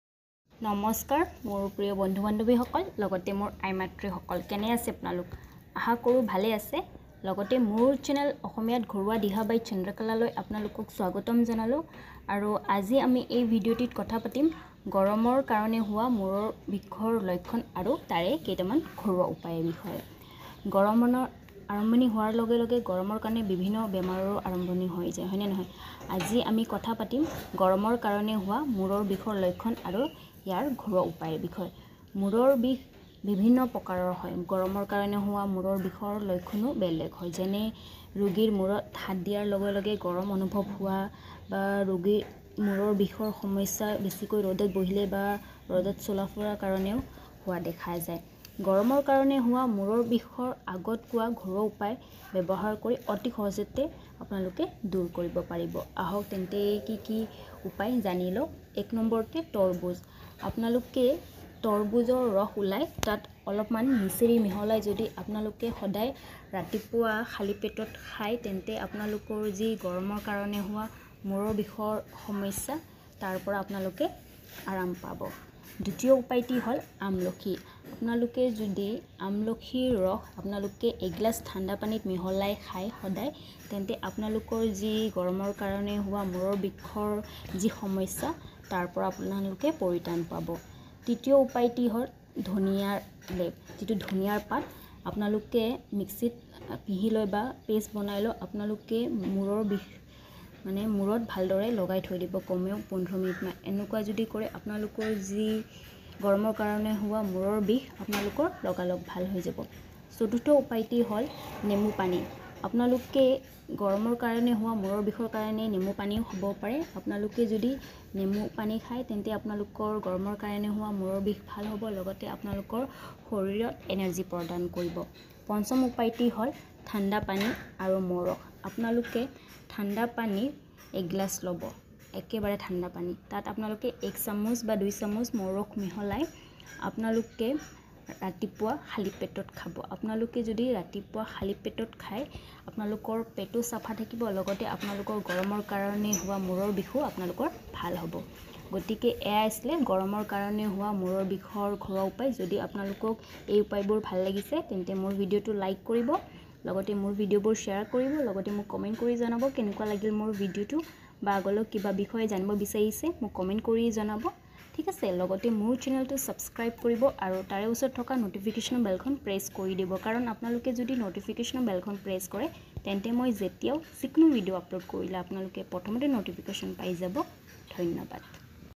গৰমৰ কাৰণে হোৱা মুৰৰ বিষৰ ঘৰুৱা উপায়।Akhomiat gharua diha by chandrakala.
এই ভিডিঅটিত মুৰৰ বিষৰ ঘৰুৱা উপায়ৰ কথা কোৱা হৈছে। গৰমৰ আৰম্ভণি হোৱাৰ লগেলগে  গৰমৰ কাৰণে বিভিন্ন  বেমাৰৰ আৰম্ভণি হয়। মুৰৰ বিষ বিভিন্ন প্ৰকাৰৰ হয়। যেনে ৰোগীৰ মুৰত হাত দিয়াৰ লগে লগে গৰমৰ অনুভৱ হোৱা। আহক জানি লও  গৰমৰ কাৰণে হোৱা মুৰৰ বিষৰ ঘৰুৱা উপায়ৰ নাম-
১।তৰবুজ।
২।আমলখি।
৩।ধনীয়াৰ পাত।
৪।নেমুপানী।
৫।ঠাণ্ডা পানী আৰু মৌৰ ৰস।